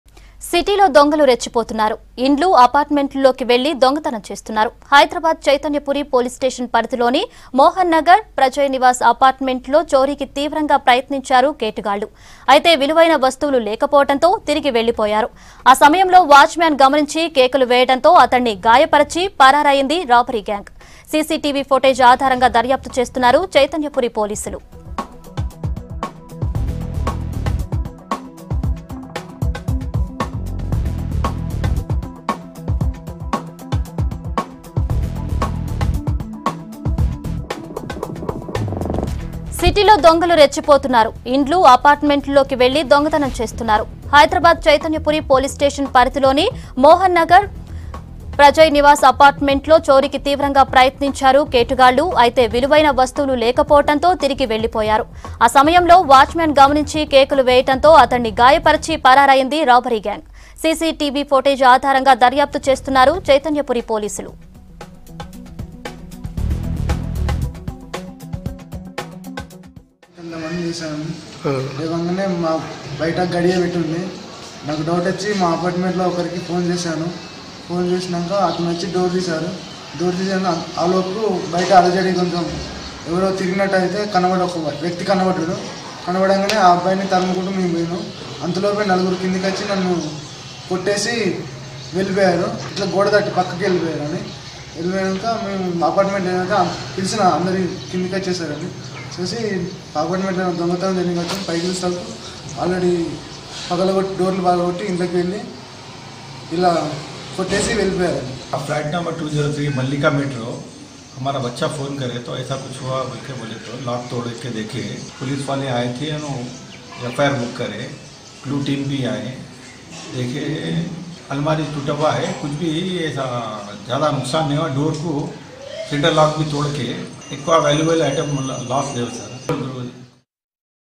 CCTV Visuals | Thieves Gang Robbery In Prajay Nivas Apartment, Mohan Nagar | Hyderabad | V6 News
CCTV Visuals | Thieves Gang Robbery In Prajay Nivas Apartment At Mohan Nagar, Attacks Watchman | Hyderabad | V6 News